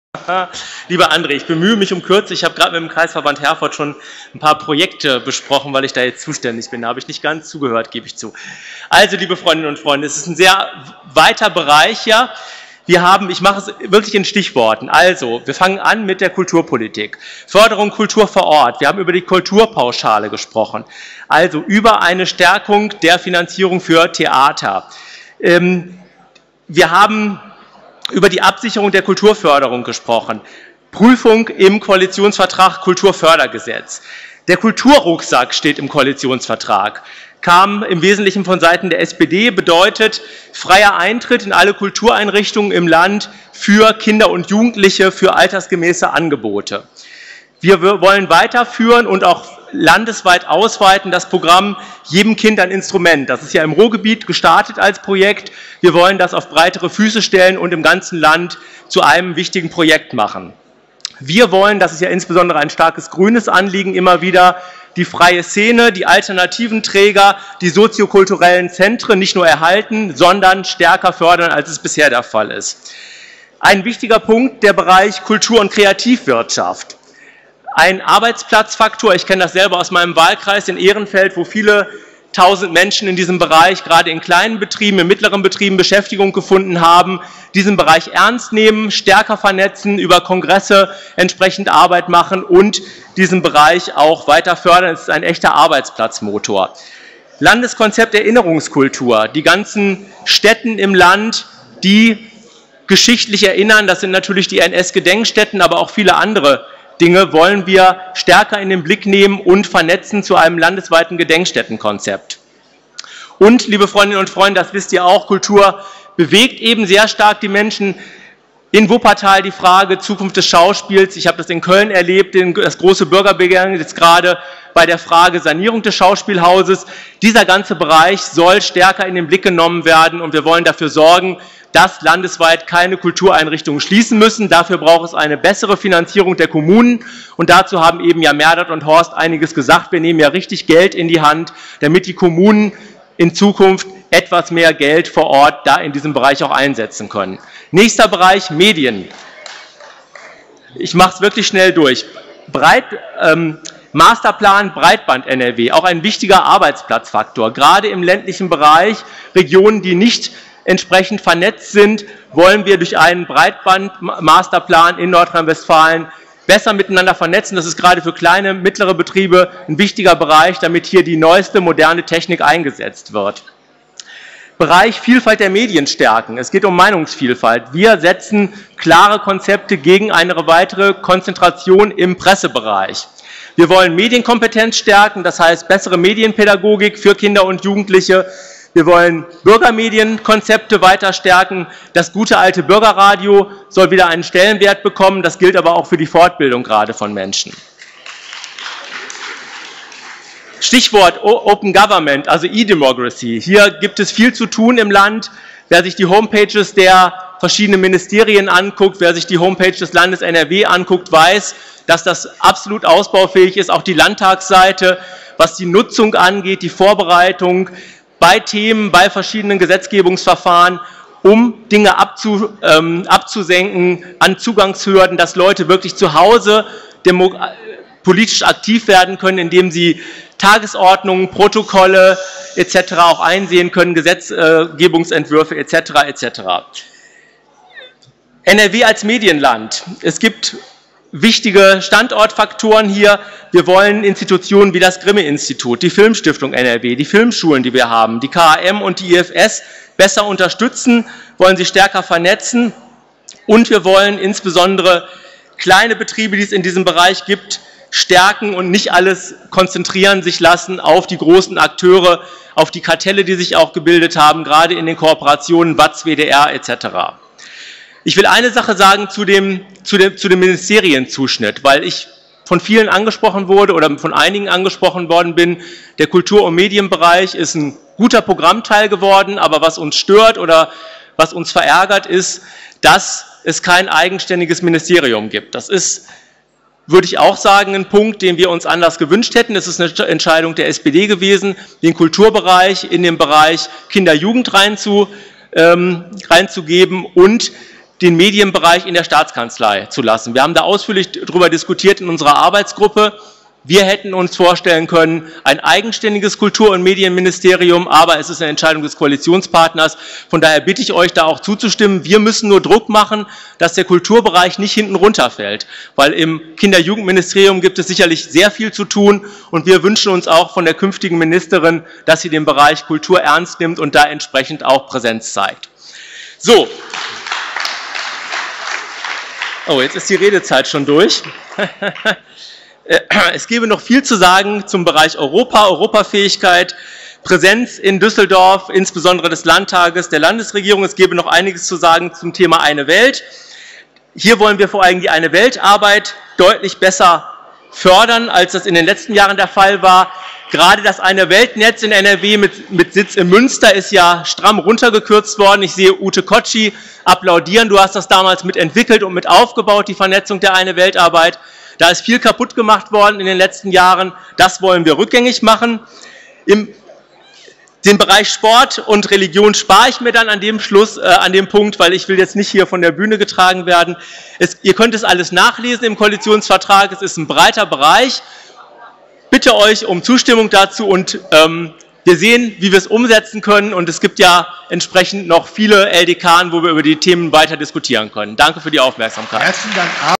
Arndt Klocke zum Kapitel Medien, Kultur
Ardnt Klocke stellt auf dem Landesparteitag das Kapitel Medien und Kultur des Koalitionsvertrags zwischen SPD und GRÜNEN vor.